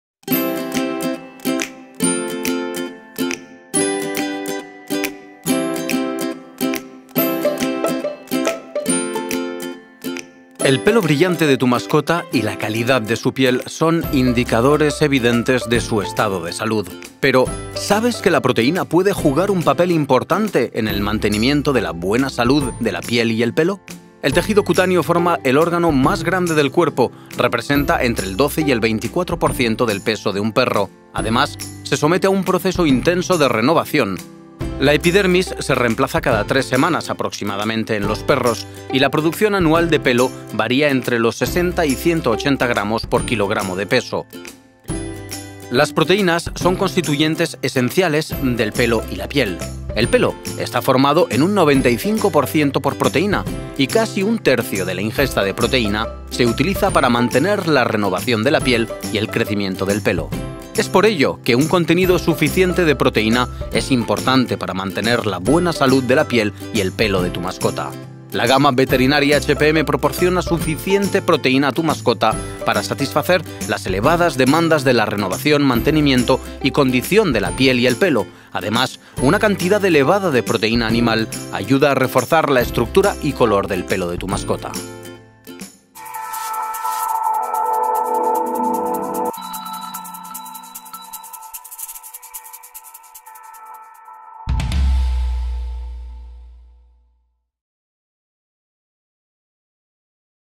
Beneficios Veterinary HPM - Soporte piel y pelo
Veterinary HPM brinda un perfil de prevención global a tu mascota. Descubre cómo mejora su piel y su pelo.